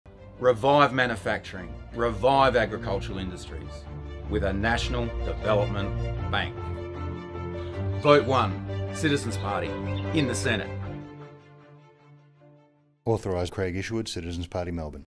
Russell Francis – Citizens Party SA Senate Candidate – For a national development bank
To revive manufacturing and agriculture industries we need low interest long term credit through a national development bank.

Policy details:
Establish a national development bank to provide long-term, low-interest credit on flexible terms for Australian manufacturing and agricultural industries. To revive manufacturing, the development bank will back local innovations that too often are lost offshore, so they are developed and manufactured domestically. For agriculture, it will support family farms through the ups and downs of seasons and markets and with their investment needs. The development bank will be a repository of financial, industrial, and trade expertise and advice for the government and industries. Expanding manufacturing and agricultural industries will require a concerted upgrade of skills training and technical education services.